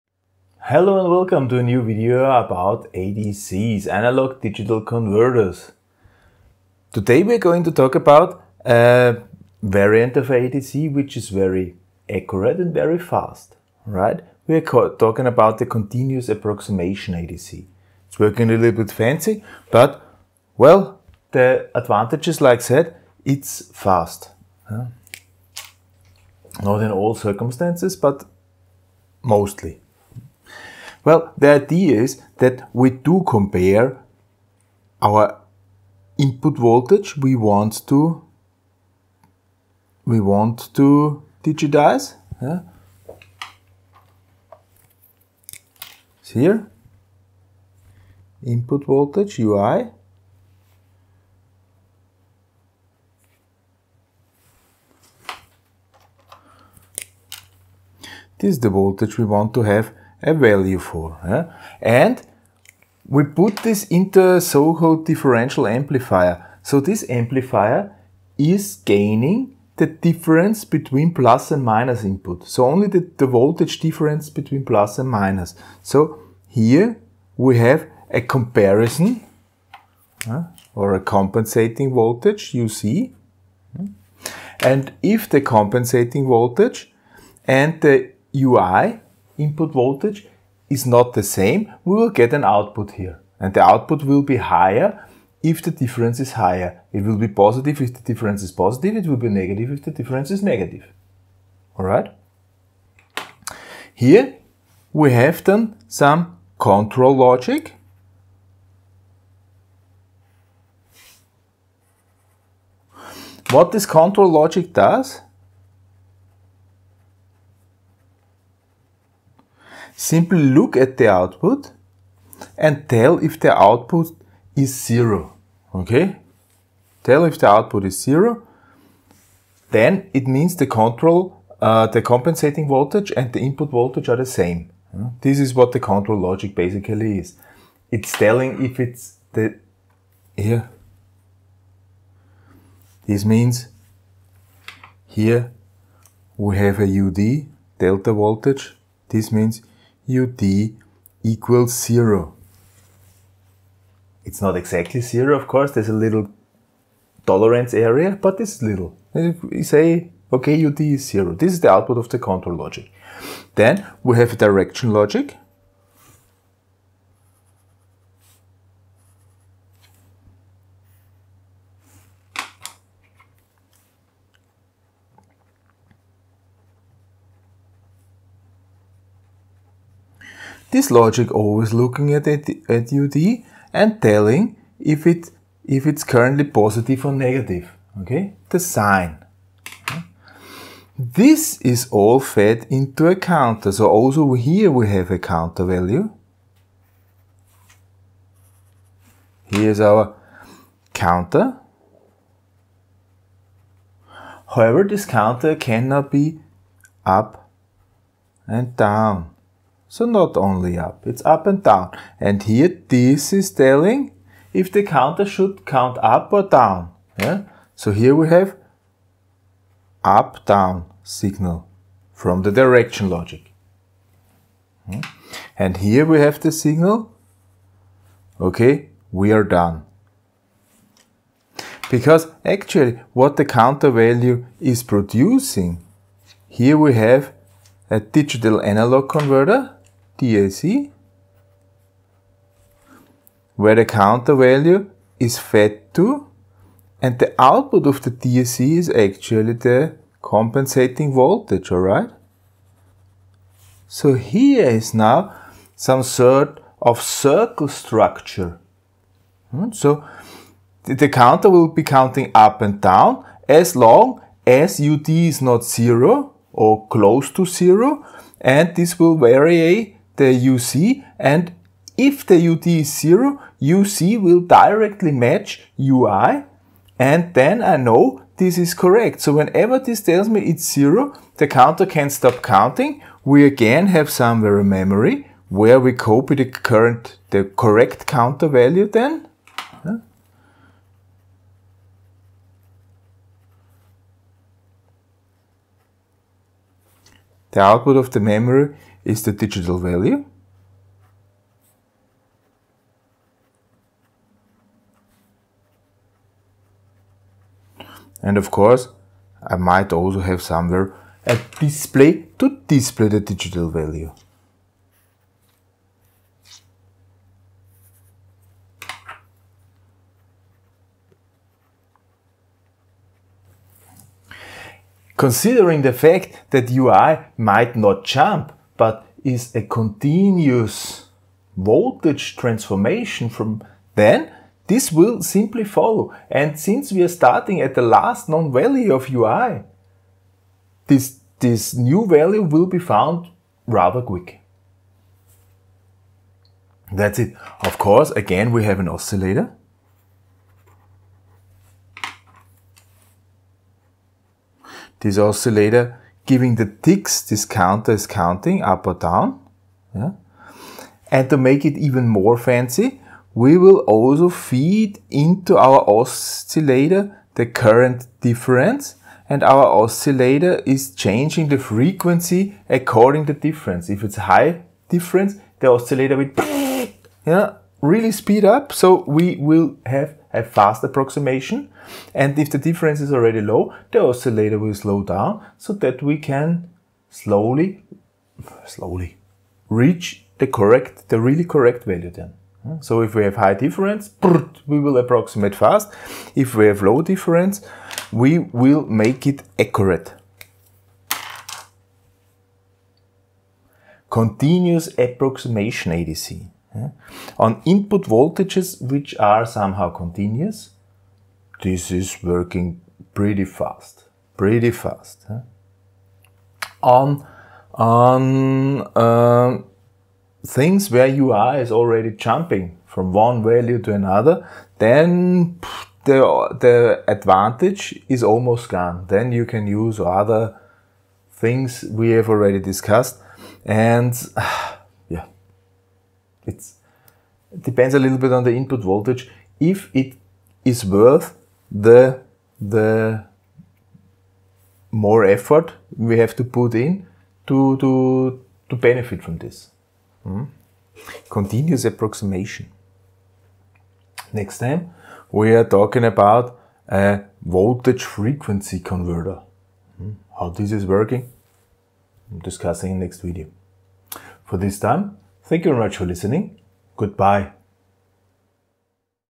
DT 44: Continuous Approximation ADC
A working principle of an Analog Digital Converter (ADC): The Continuous Approximation Methode.